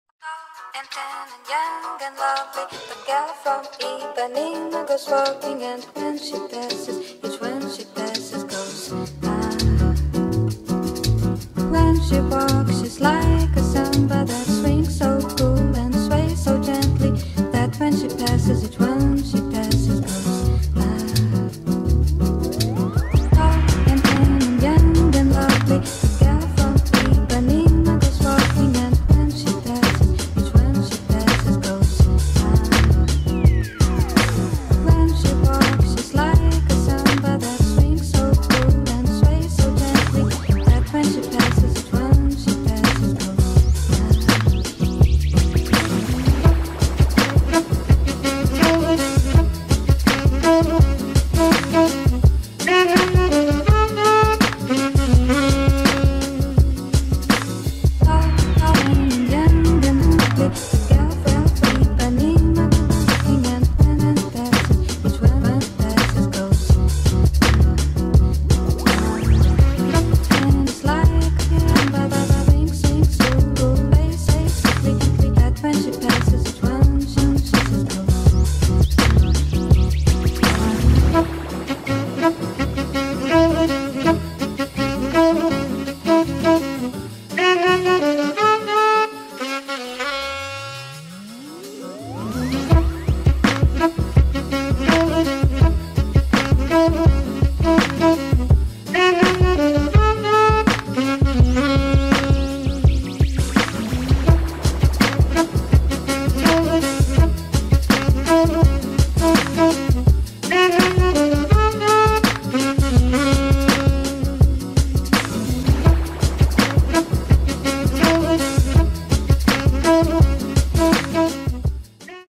LifestyleHotels: Belmond Copacabana Palace
O Belmond Copacabana Palace é o hotel mais icônico do Rio de Janeiro, recebendo hóspedes do mundo inteiro e visitas ilustres de alguns famosos, que são imortalizadas em seu Hall of Fame. Descubra todos os detalhes da experiência de hospedagem nesta sofisticada propriedade em nosso site!